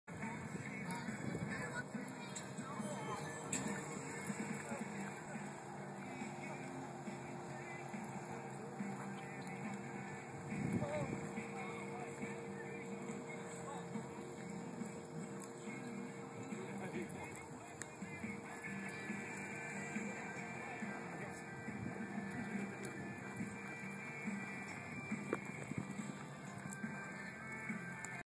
Flippin Gold Money pole bending
Flippin Gold Money, after just a few practice sessions on the poles, we cruised through at the local playday with a low 24, fastest time of the night...not a bad start!